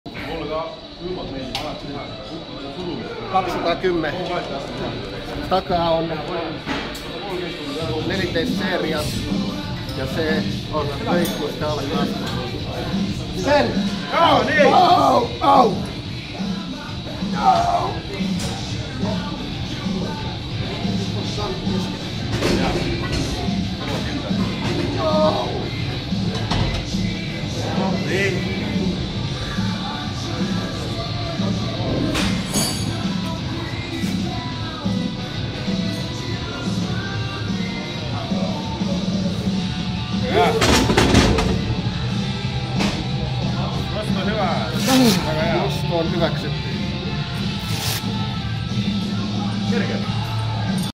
Bench Press raw 210 kg - Timo Majuri - HC GYM
Preparing to WPC Europeans in Riga 2015 - 9 days left